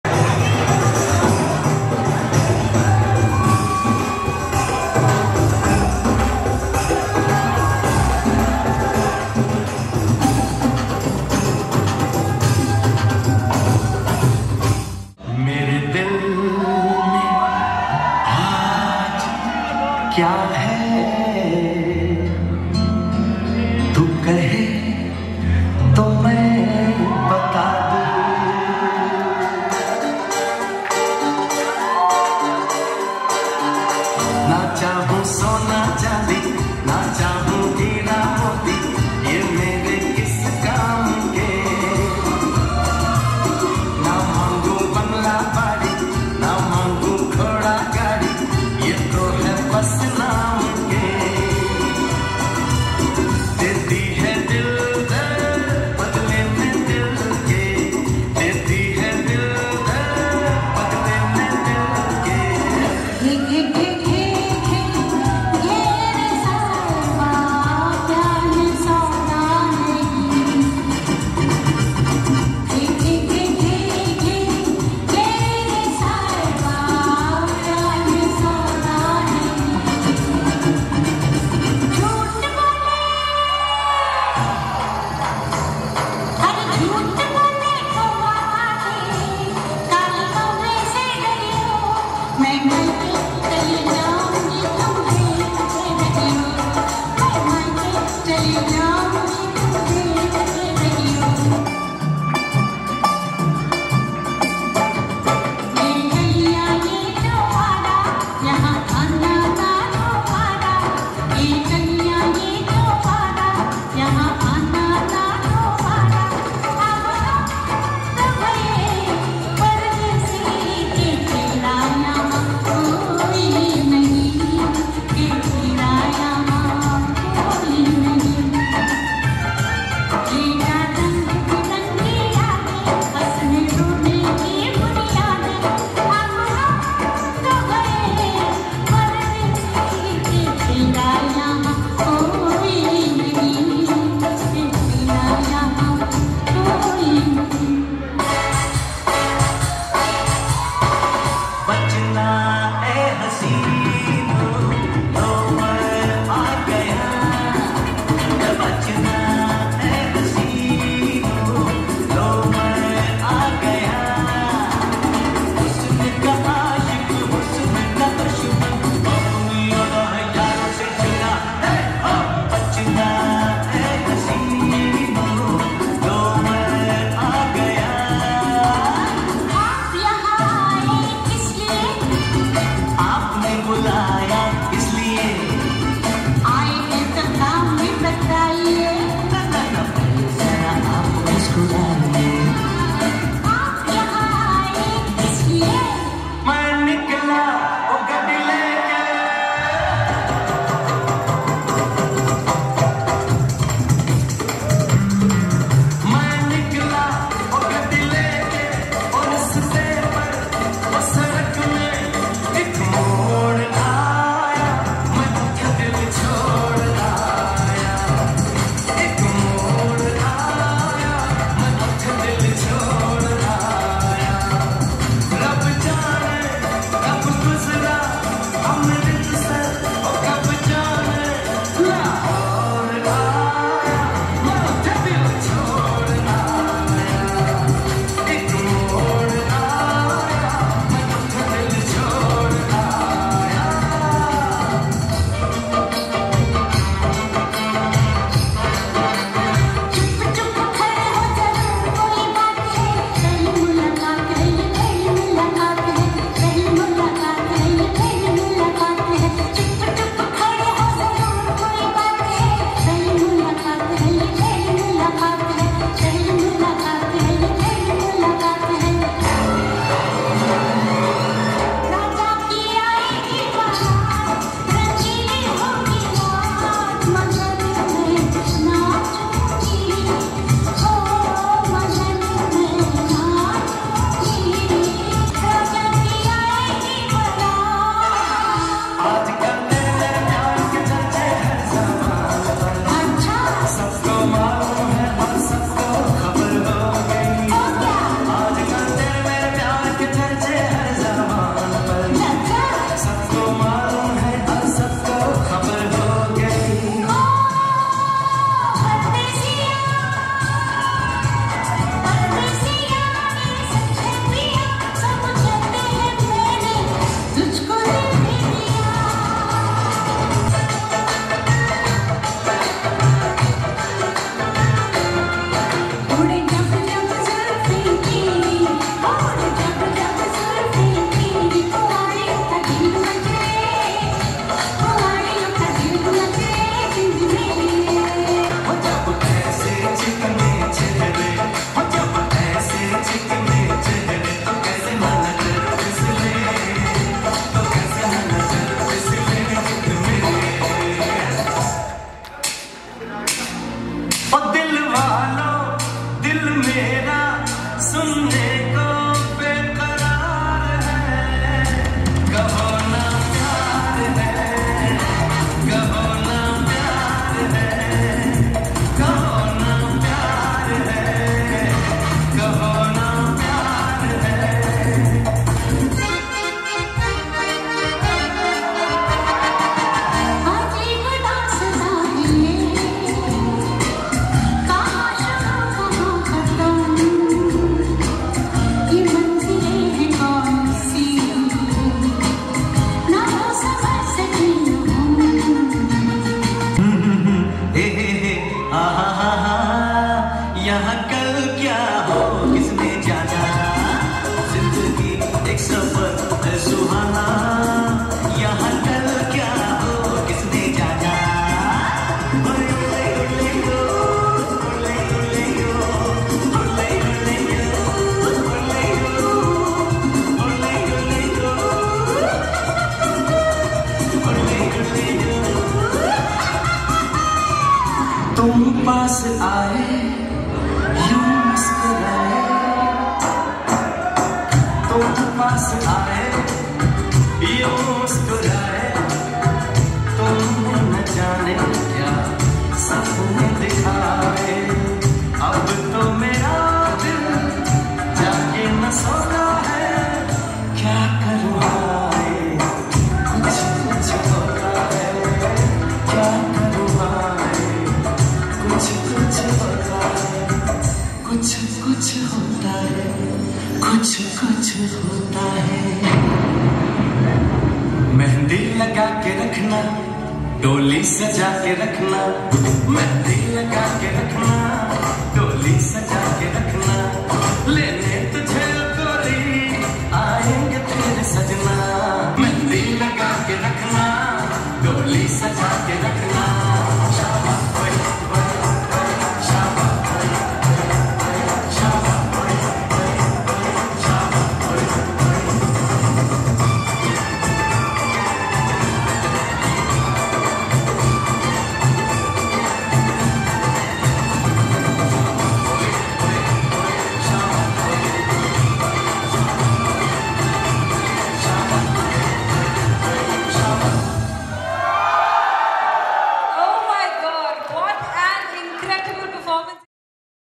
Family Dance l Wedding Special Choreography l Dance Cover l Sangeet Special l Gem Girls
Hello guys,

We our back with another family dance video on such an amazing family special wedding mashup just for you all :)

Hope you like it! Share your thoughts with us in comment section.